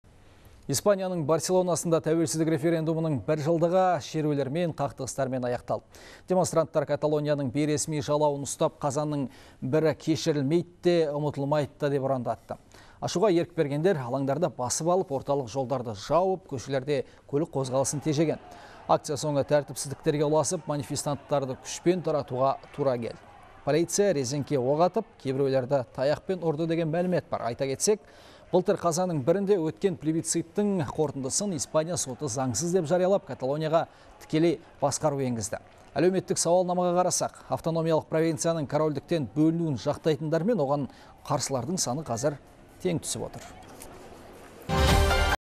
Каталония тәуелсіздігі үшін референдумның өткеніне бір жыл толды
Испанияның Барселонасында тәуелсіздік референдумының бір жылдығы шерулермен, қақтығыстармен аяқталды. Демонстранттар Каталонияның бейресми жалауын ұстап, Қазанның 1-і кешірілмейді де, ұмытылмайды да» деп ұрандатты. Ашуға ерік бергендер алаңдарды басып алып,  орталық жолдарды жауып, көшелерде көлік қозғалысын тежеген. Акция соңы тәртіпсіздіктерге ұласып, манифестанттарды күшпен таратуға тура келді. Полиция резеңке оқ атып, кейбіреулерді таяқпен ұрды деген мәлімет бар.